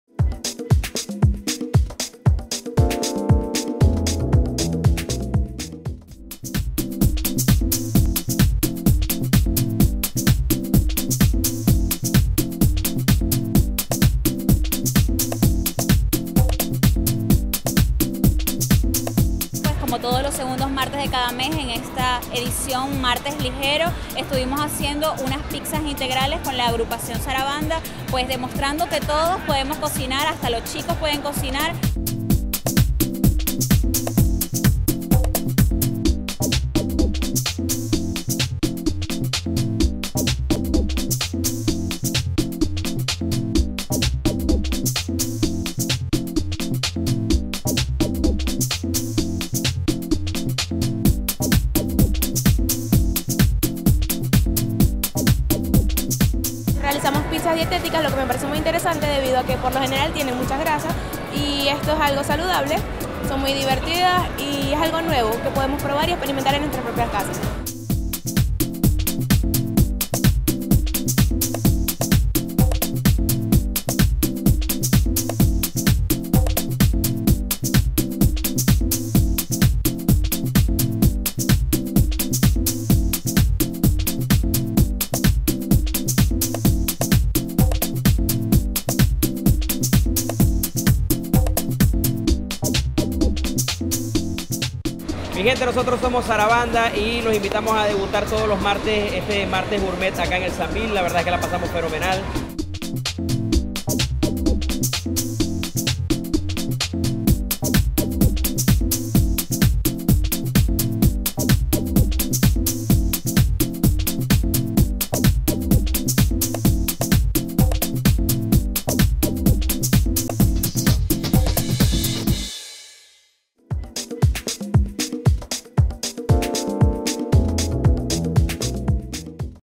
Martes Gourmet con Sarabanda SAMBIL Maracaibo 2012
Todos los segundos martes de cada mes acompaña a Mariangel Ordoñez a preparar deliciosas recetas que además son fáciles de hacer y son saludables. En esta ocasión contamos con la participación de la agrupación musical marabina Sarabanda, quienes prepararon unas pizzas integrales.

Disfruta de esta y muchas otras actividades que te ofrece SAMBIL Maracaibo totalmente gratis!